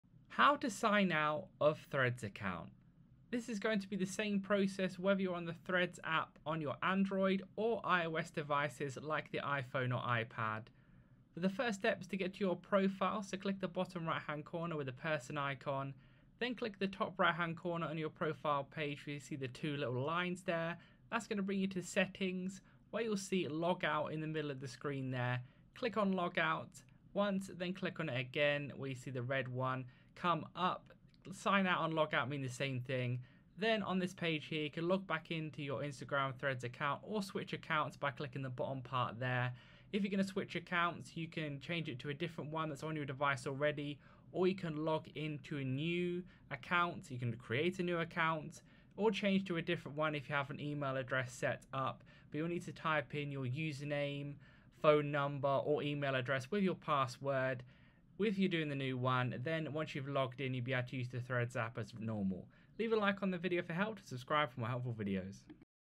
How to Sign Out of Threads Account
How to Sign Out of Threads Account. You can log out from threads with android or ios devices like the iphone or ipad. Also, you can log out in instagram threads on your laptop computer with the new instagram threads app update in 2023.